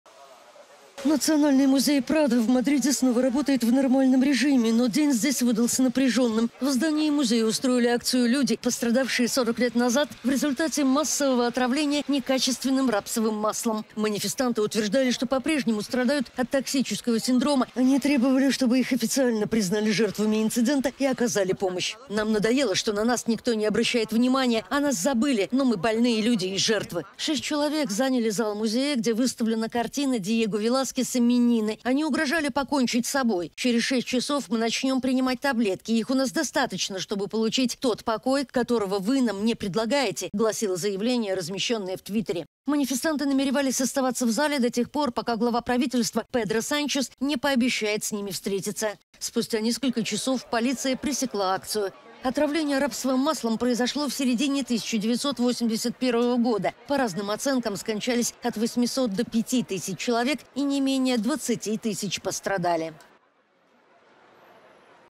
Пострадавшие от отравления рапсовым маслом устроили протест в музее Прадо
"Нас забыли, но мы - жертвы"